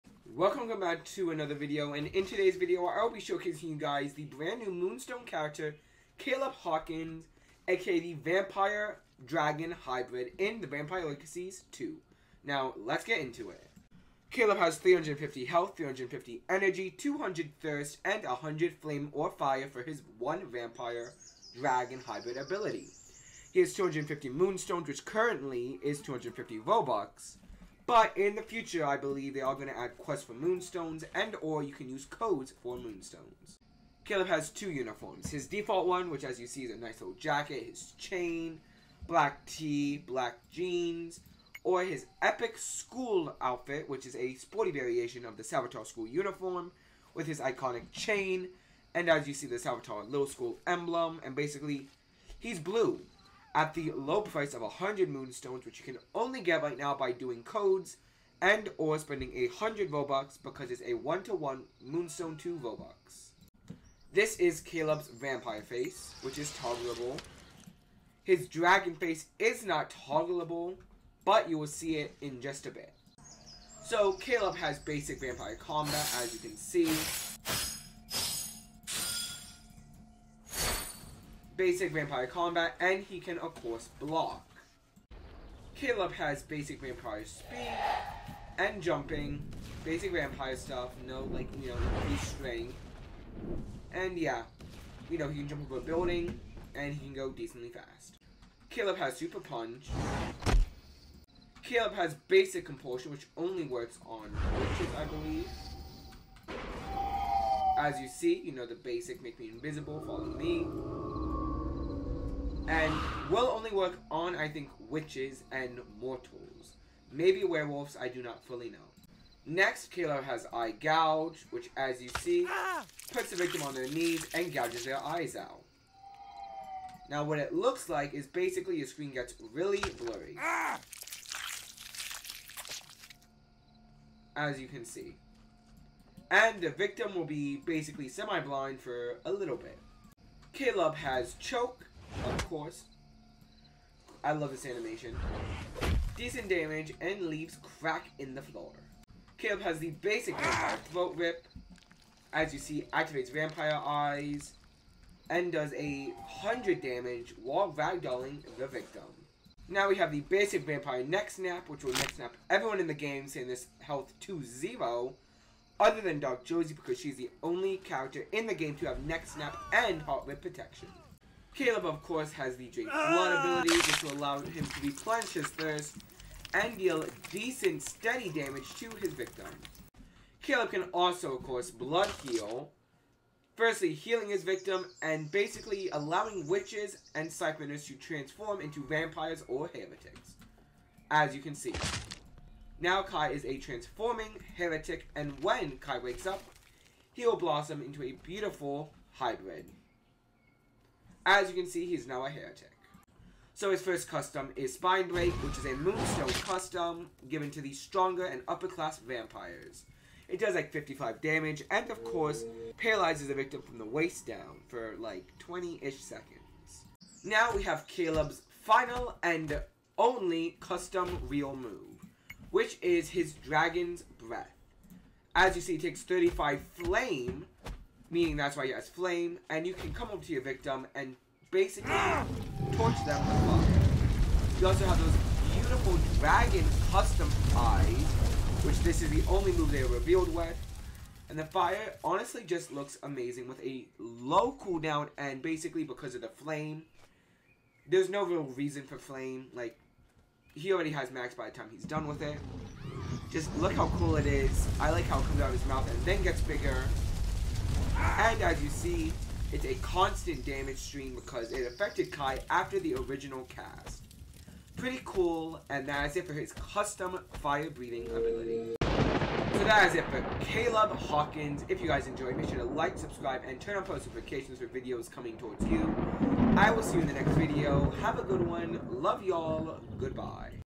🩸🧛🩸Kaleb Hawkins🩸🧛🩸 the 🐲🔥🐉Dragon Hybrid🐲🔥🐉
Kaleb Timestamps

0:00-0:13 Intro
0:14-0:33 Stats
0:34-1:03 Outfits
1:04-1:13 Vampire Face Reveal
1:14-1:25 Combat
1:26-1:38 Vampire Speed and Jump
1:39-1:41 Super Punch
1:42-1:59 Compulsion
2:00-2:20 Eye Gouge
2:21-2:29 Choke
2:30-2:38 Neck Snap
2:39-2:59 Drink Blood
3:00-3:32 Blood Heal
3:33-3:36 (MOON STONE EXCLUSIVE) Spine Break
3:37-4:28 Dragon Breath
4:29-4:40 Outro